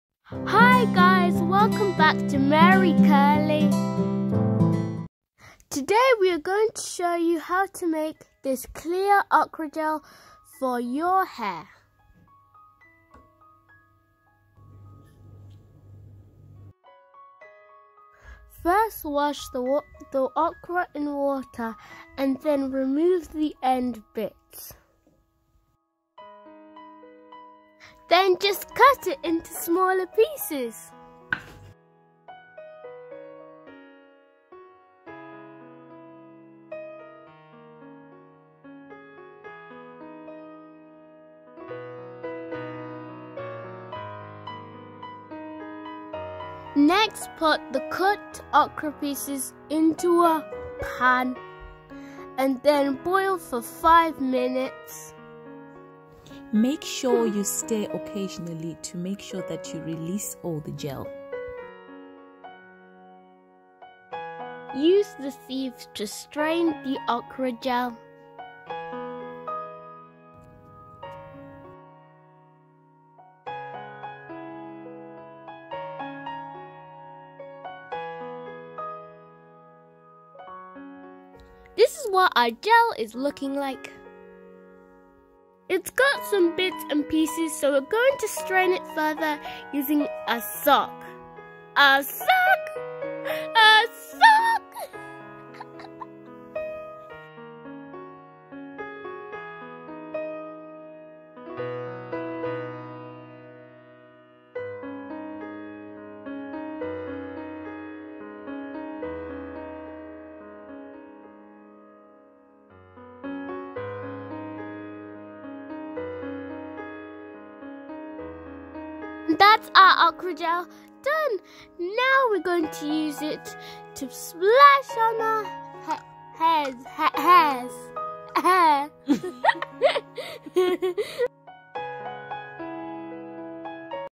DIY OKRA GEL 4 NATURAL HAIR STYLING, MOISTURISING & DETANGLING GROW natural hair non flaking
Okra gel is great for styling, moisturising and detangling natural hair 😍

okra gel does not cause flaking like other gels do. Its 100% natural and packs a lot of nutrients and vitamins which benefit the hair.
Its very easy and straightforward to make. Store in the refrigerator and use within a  week.

Oils of choice can also be added to the okra gel.

Merry Curly _ keeping it natural.